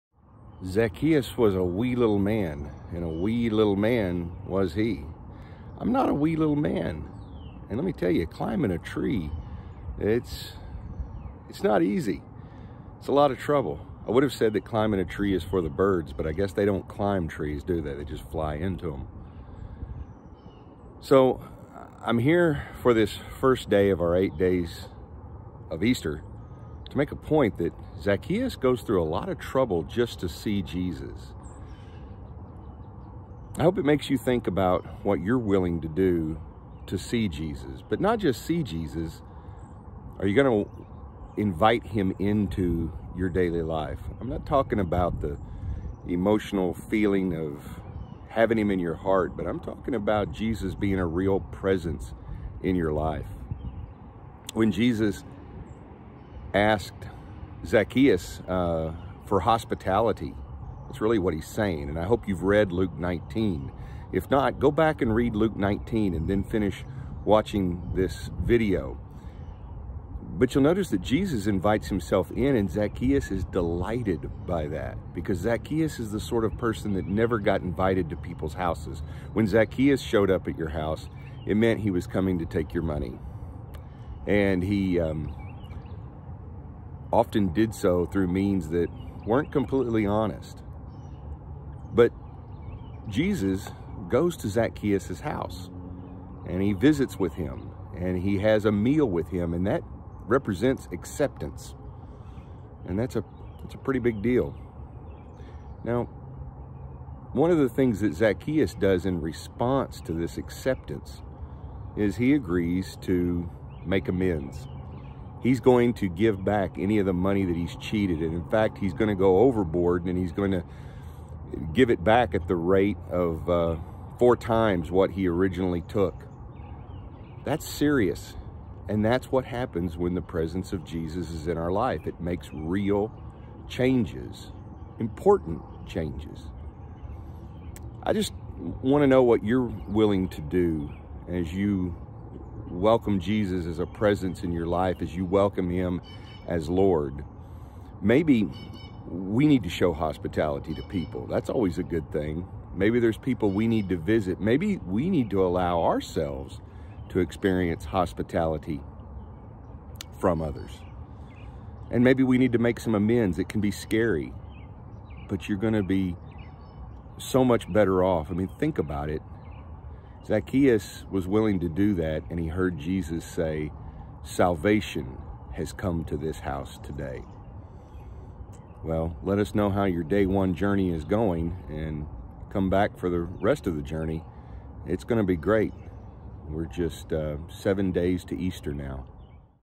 Day 2 - Eight Days of Easter
What are you willing to do to welcome Jesus into your life?

Yesterday, the West-Ark Church of Christ began an eight day spiritual journey leading up to Easter.  Everyone is welcome to check this channel and join us in the daily reflections and activities.